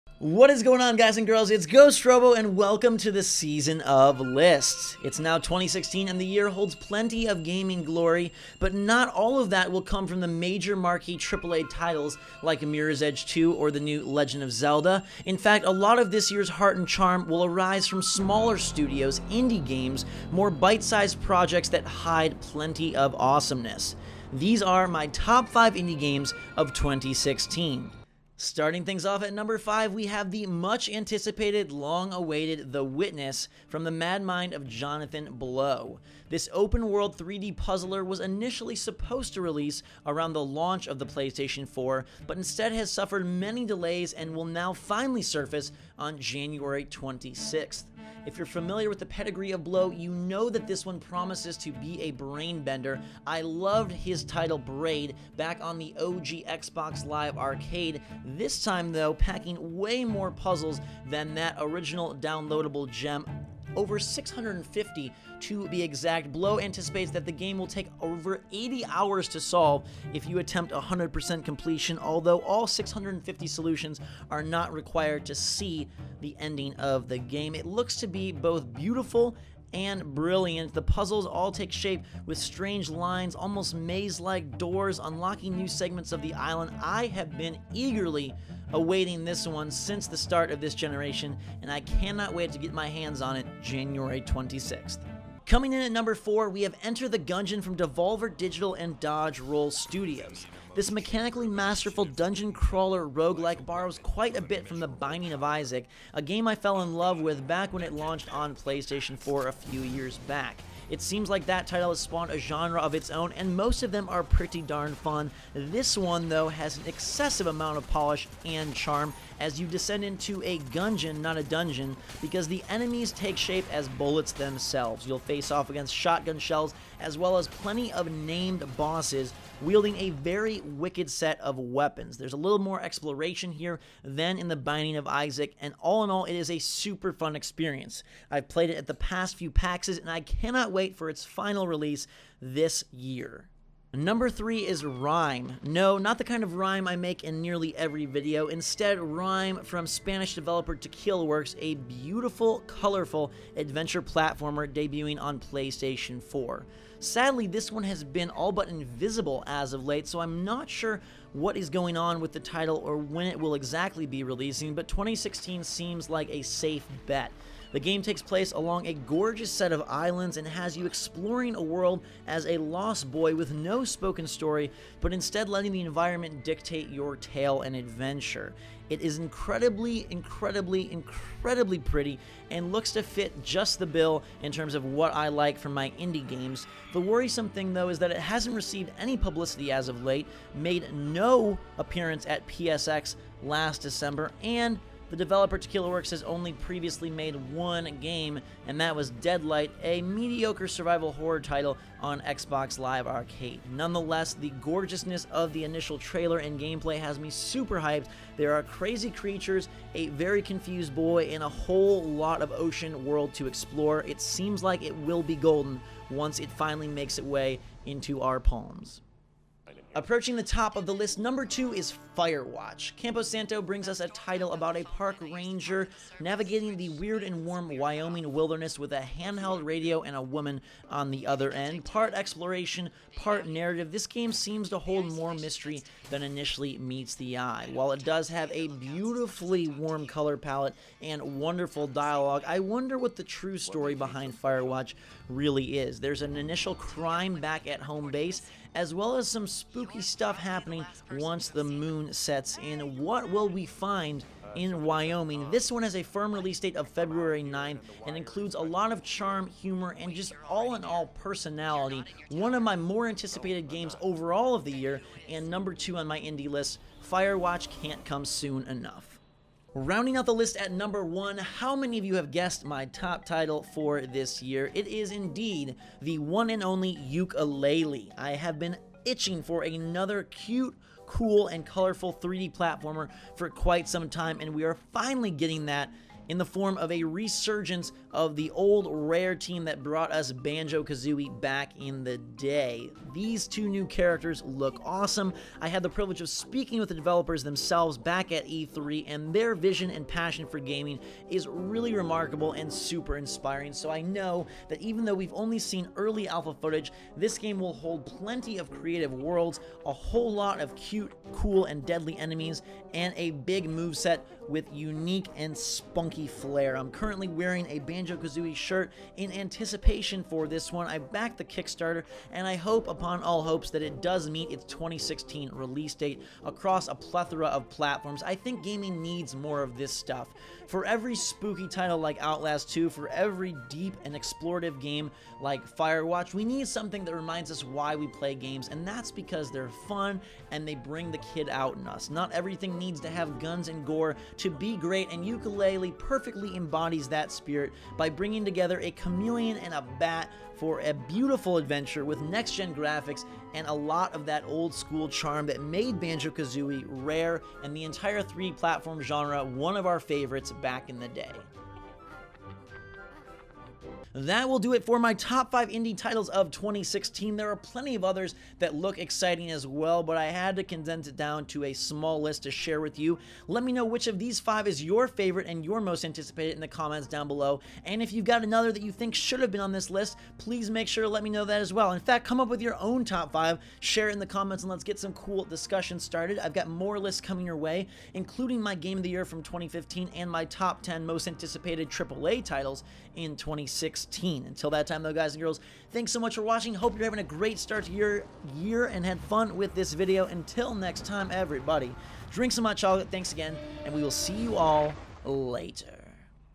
TOP 5 INDIE GAMES OF 2016!!
The Top 5 Indie Games of 2016!! Which games will be the best, which are we looking most forward to, and which will be the TOP Indie Game of 2016?? Give me your list in the comments below :D

GhostRoboArmy for life!!